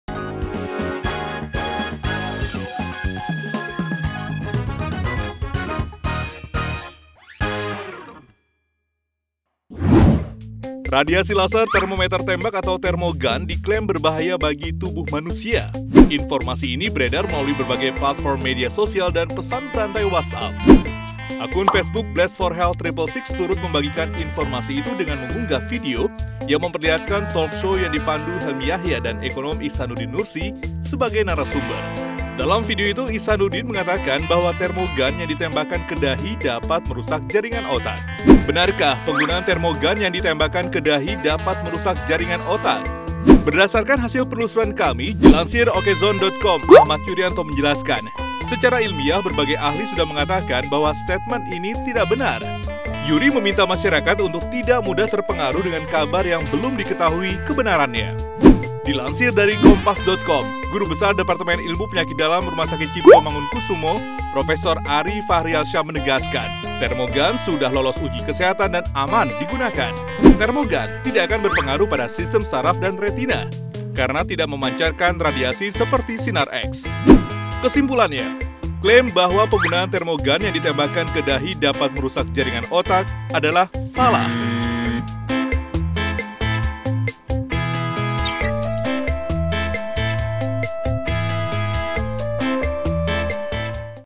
[Ceki-ceki] Benarkah Radiasi Thermo Gun Bisa Merusak Otak? Ini Faktanya
Radiasi laser termometer tembak atau thermo gun diklaim berbahaya bagi tubuh manusia. Benarkah info tersebut?

Cek faktanya di video berikut!